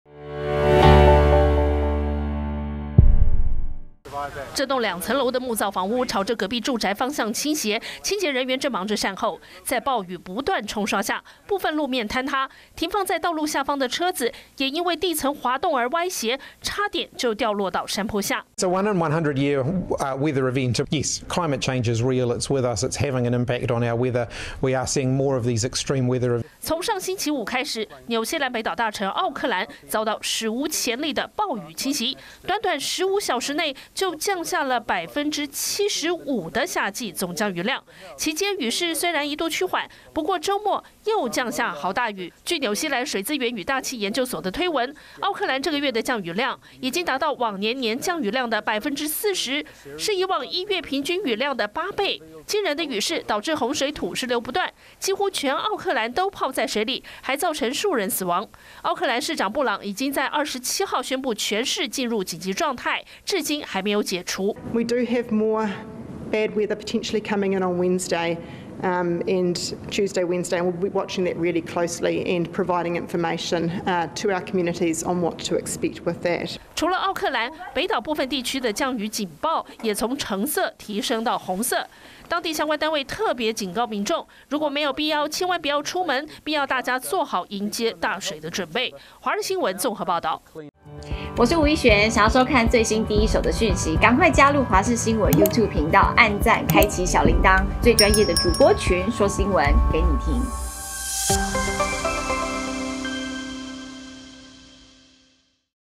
紐西蘭破紀錄暴雨 土石流洪災釀4死｜華視新聞 20230131
紐西蘭北島大城奧克蘭，當地時間27日遭史上最大暴雨侵襲。儘管雨勢一度趨緩，但周末又再降下豪雨，引發土石流洪水等災情，造成4人喪生。就在大家開始善後時，紐西蘭氣象單位警告，當地周二跟周三，又會有另一波降雨，要民眾提高警覺。目前奧克蘭全市仍維持緊急狀態，新任上的紐西蘭總理希金斯說，這場暴雨證明了氣候變遷是真實存在，並正在影響紐西蘭的天氣。